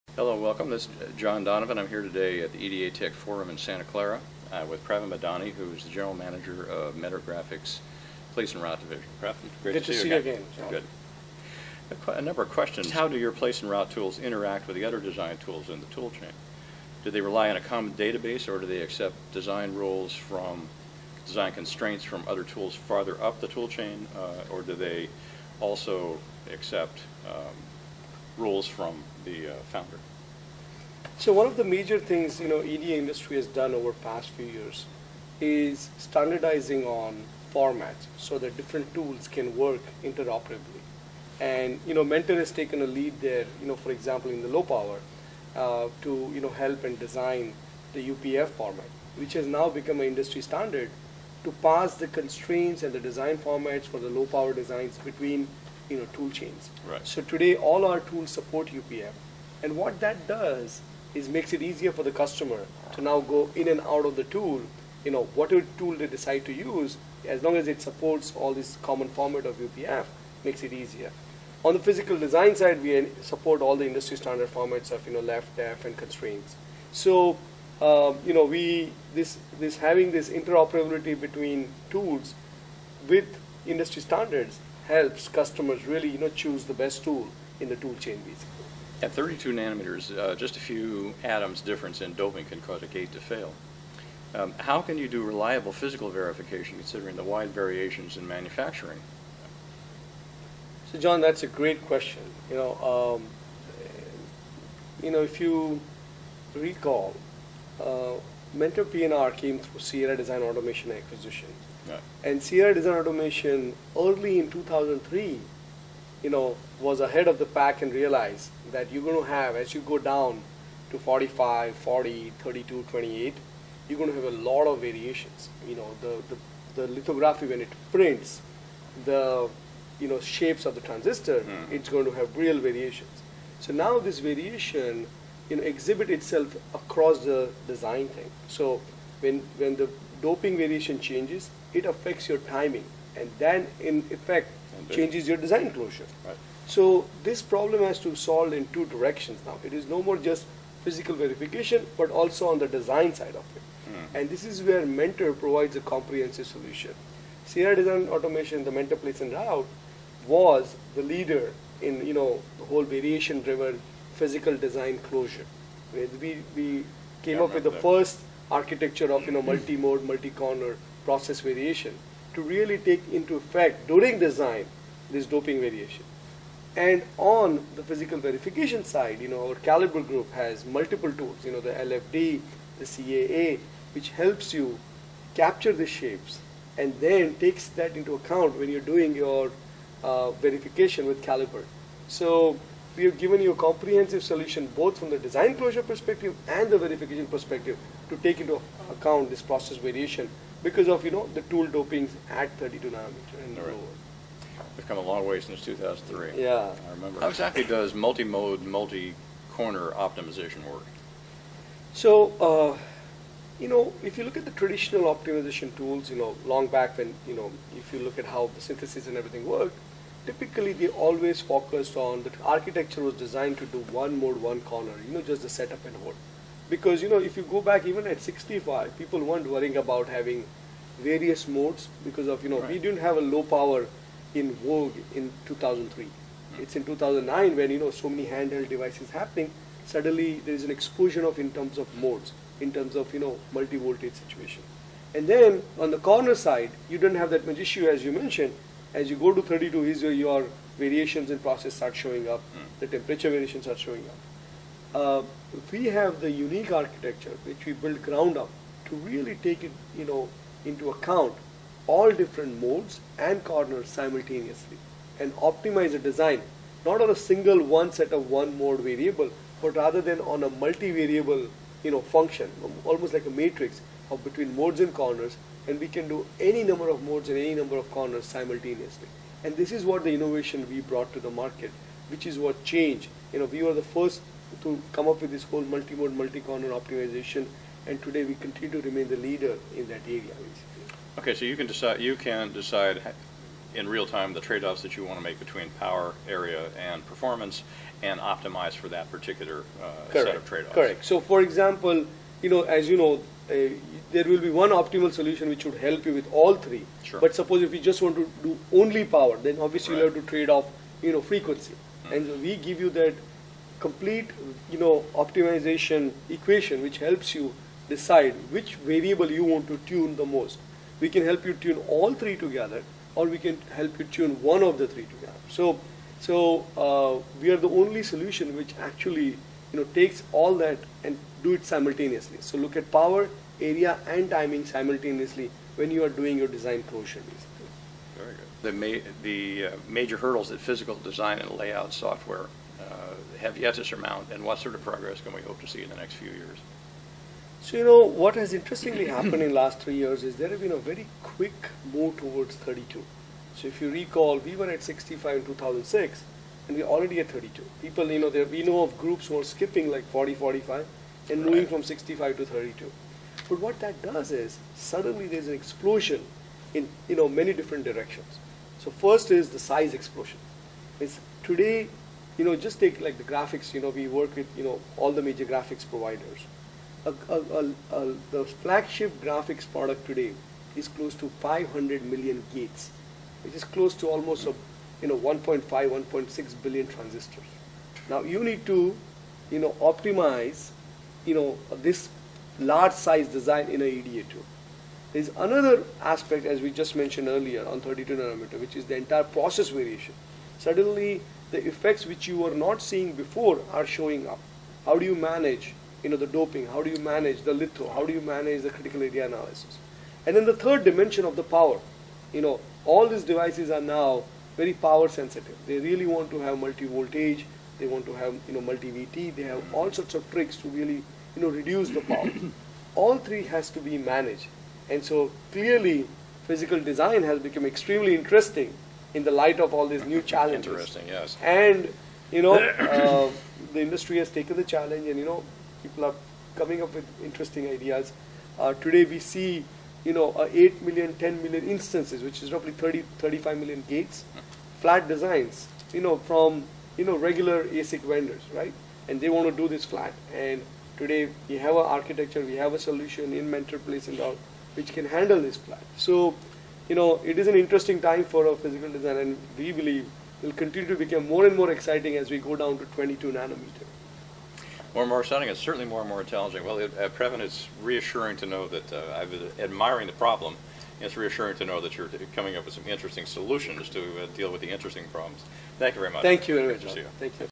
Pravin Madhani EDA Tech Forum Sept 09
Interview with Pravin Madhani, GM of Mentor Graphics Place and Route Division, by John Donovan, Editor/Publisher of Low-Power Design Conducted at the EDA Tech Forum in Santa Clara, CA, September 9, 2009.